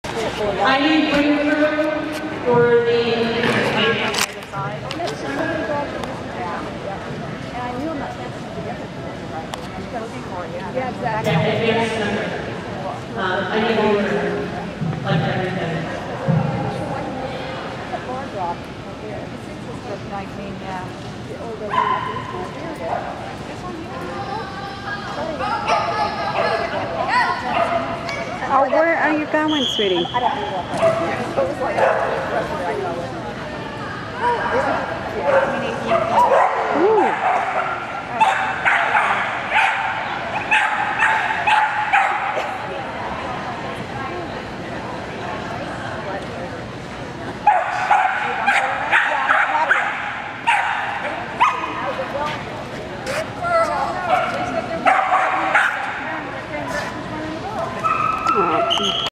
Smitty GP Round 2 Perry 2009
Wild little woman...I'm guessing she didn't know it was the seesaw, and then told me ALL about it!  I finally had to take her off, as she was just a little spun up!  Baby dog!